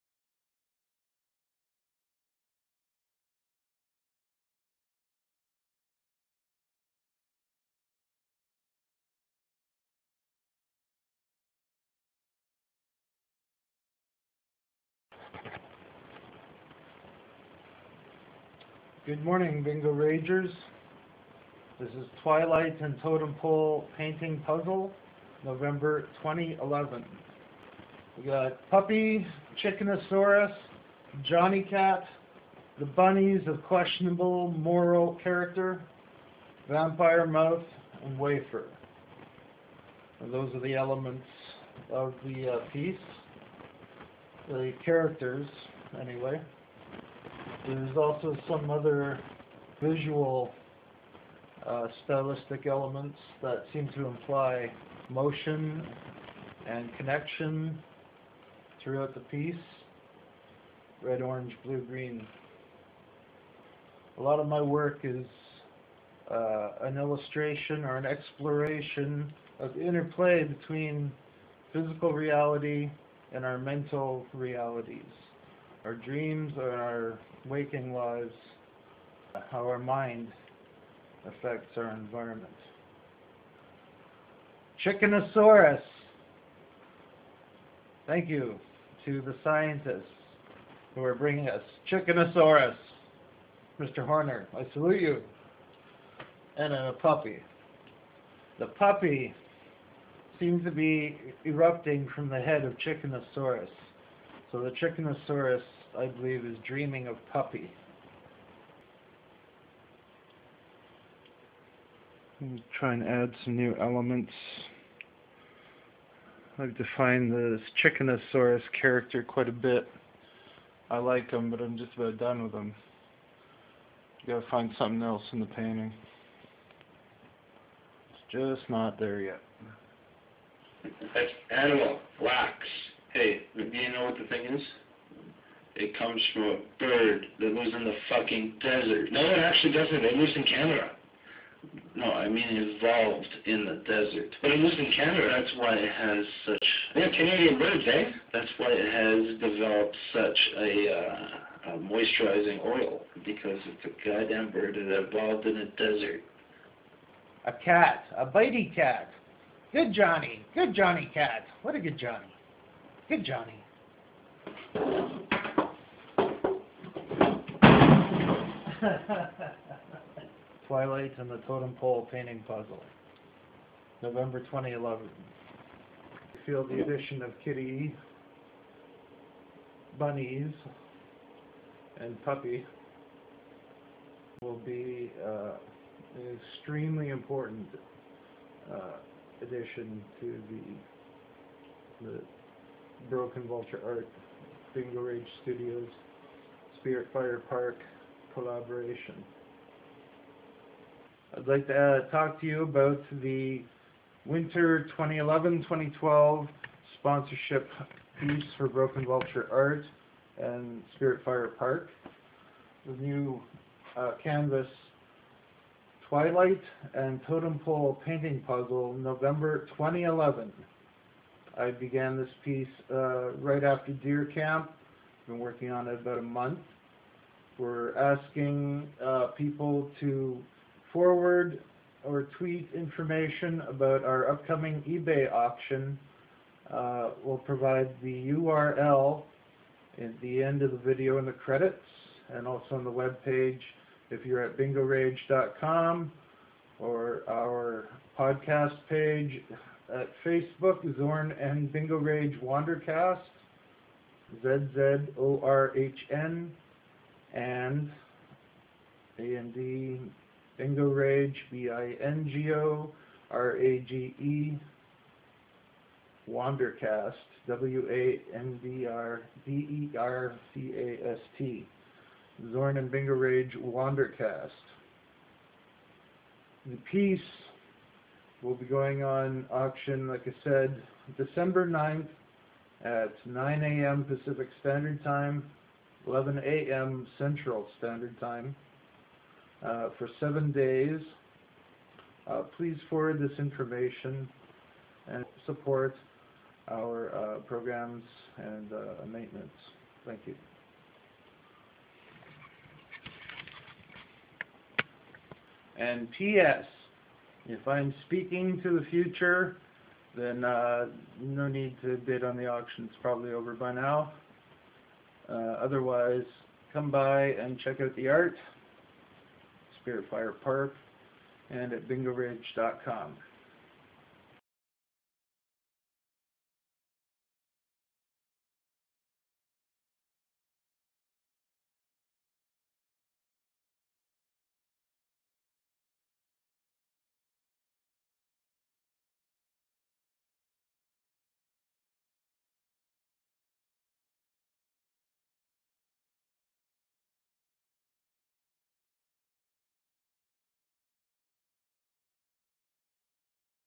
TwilightAndTotemPolePaintingPuzzle_Nov2011
The Winter 2011/12 Sponsorship piece for Broken Vulture Art and Spirit Fire Park. This work will be available for auction at Ebay for 7 days, beginning December 09, 2011. It is approx. 50 inches high x 22 inches wide x 4 inches deep; stretched canvas on a 1"x4" boxframe. Inspirations include: chickenosaurus, pop-vampirism, brave JohnnyCat and dubious bunnies.